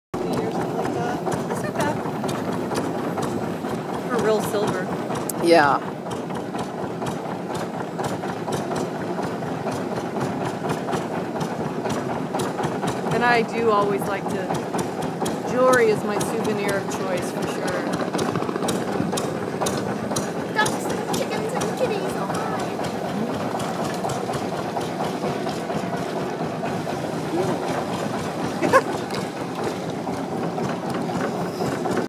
Boating on Inle Lake, Myanmar
Inle Lake is a fairly shallow lake in Myanmar, and has floating gardens and houses built on stilts. We took a boat ride on a beautiful day.